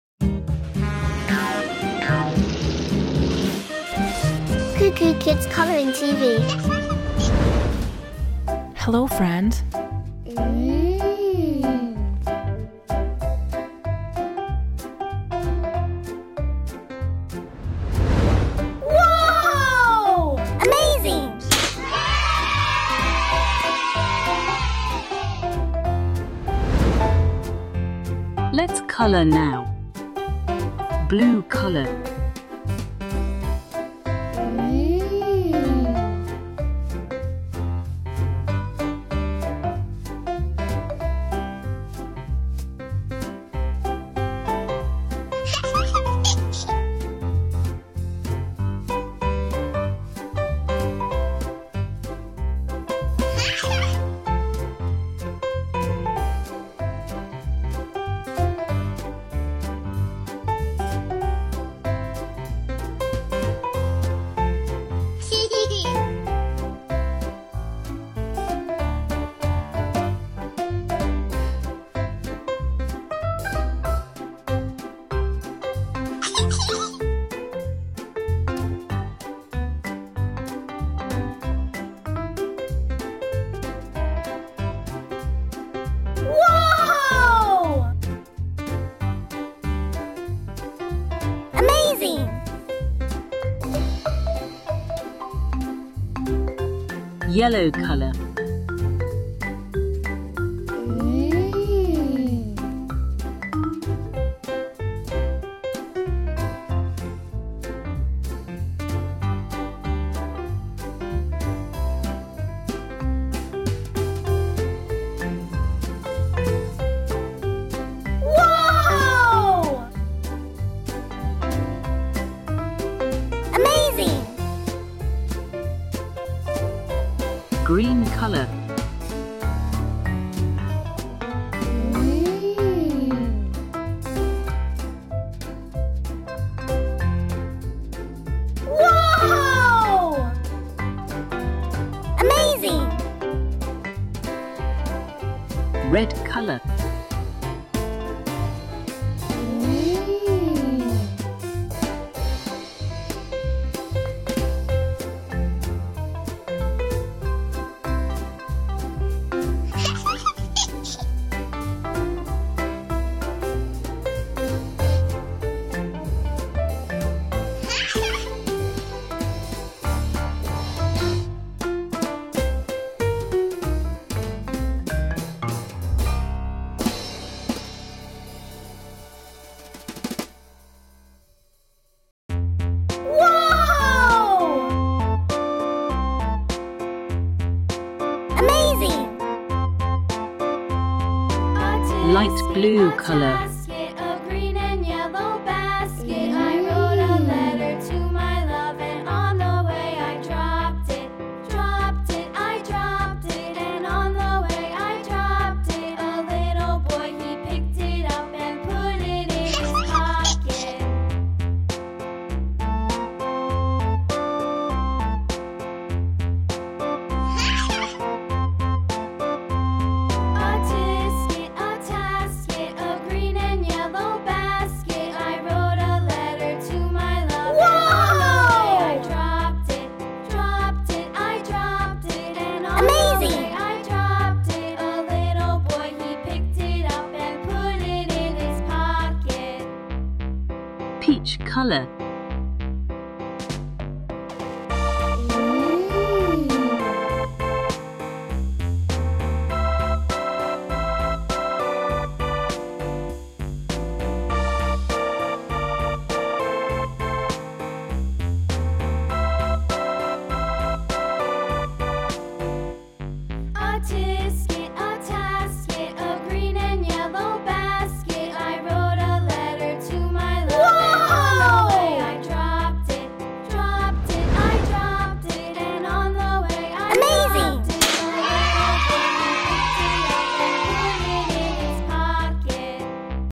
Little Boxer Painting and Coloring for Kids, Toddlers | How to Paint, Color Boxer | Video # 27
"Little Boxer  Painting and Coloring for Kids, Toddlers | How to Paint, Color  Little Cat Easy" is a creative activity designed for young children to engage in imaginative play and enhance their artistic skills.

This activity involves painting and coloring Little Boxer -themed image using simple and easy-to-follow techniques. Kids will learn how to color and paint Little Boxer, creating colorful and eye-catching artworks that will inspire their creativity and imagination.

Overall, this activity offers a fun and engaging way for children to express themselves through art while also developing their fine motor skills, color recognition, and imagination. It is an excellent choice for parents, teachers, and caregivers who want to encourage children to explore their creative side while having fun with Little Boxer.